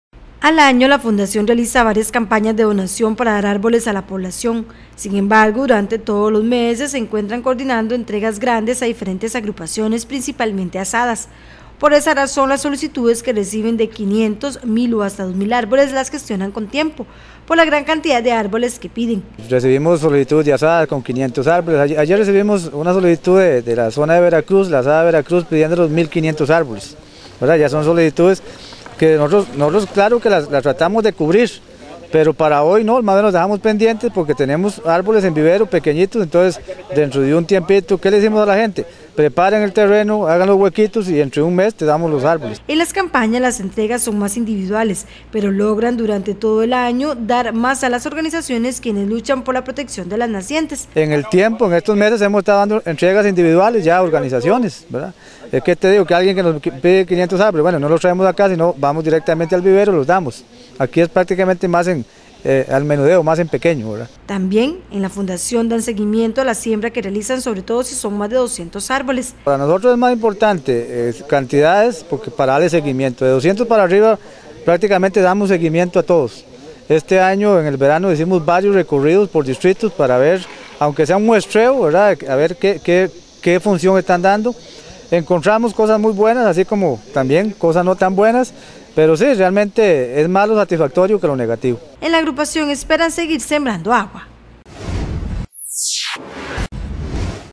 Fundación Sembrando Agua da constantemente árboles a agrupaciones
Al año, la Fundación realiza varias campañas de donación, para dar árboles a la población, sin embargo, durante todos los meses, se encuentran coordinando entregas grandes a diferentes agrupaciones, principalmente Asadas.